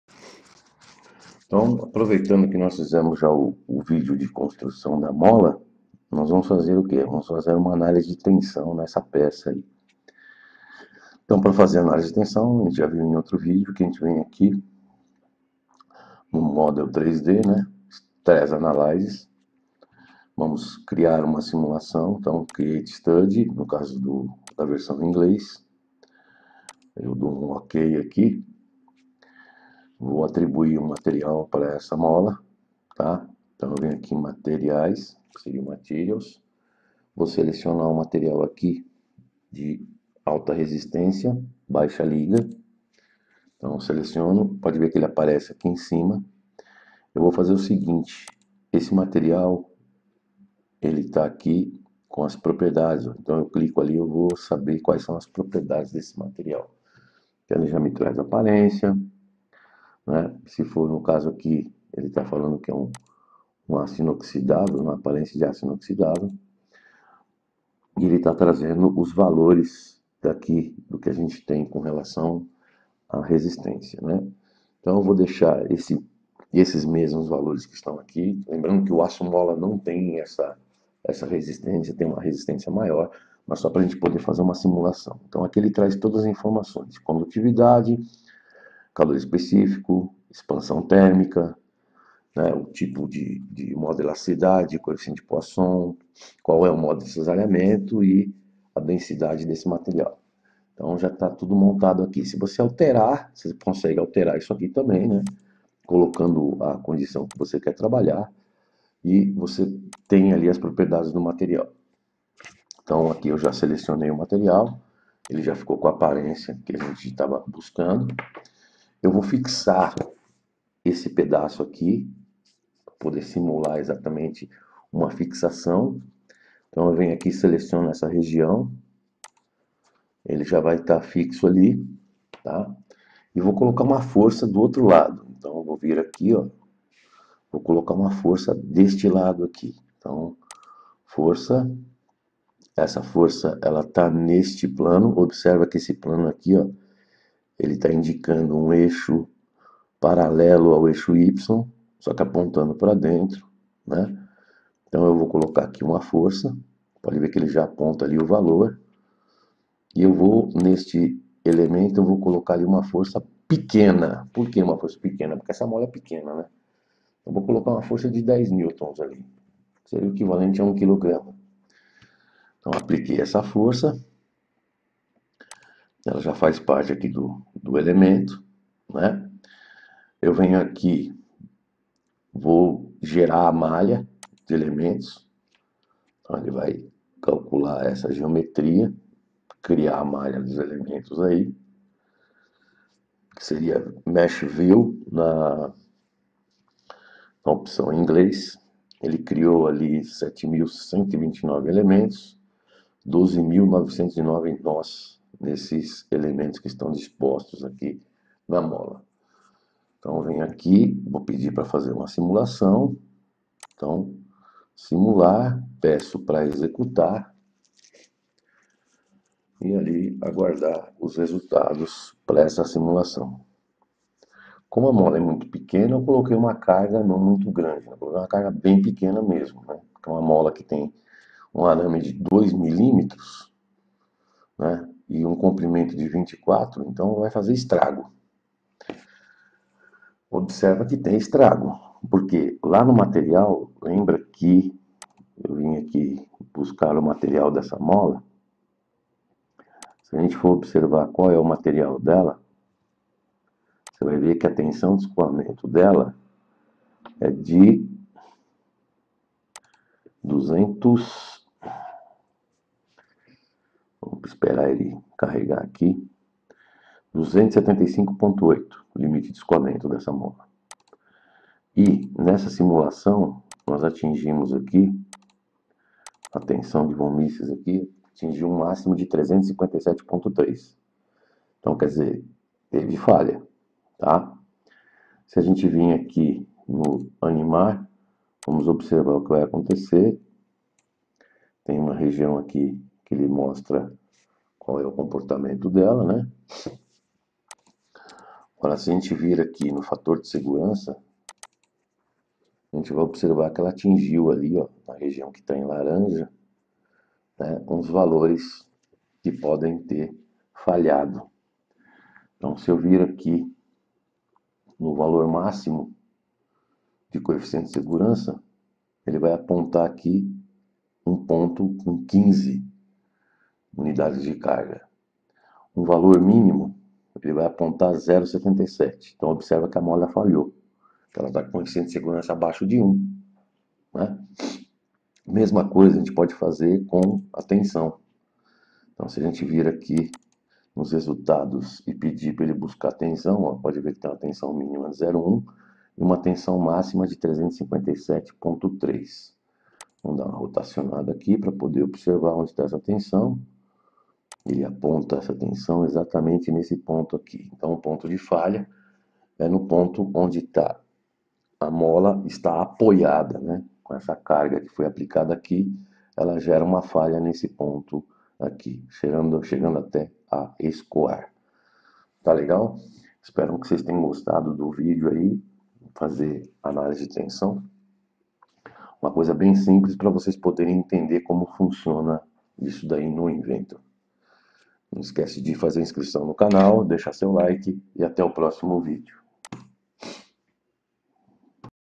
Curso Autodesk Inventor Análise de tensões em uma mola de compressão aula 8
Neste vídeo é utilizada a ferramenta de análise de tensões do software Autodesk Inventor para simulação de tensões em uma mola de compressão, mostrando todos os parâmetros de configuração para a realização da análise. Definir materiais, restrições, carga e geração da malha de simulação por elementos finitos.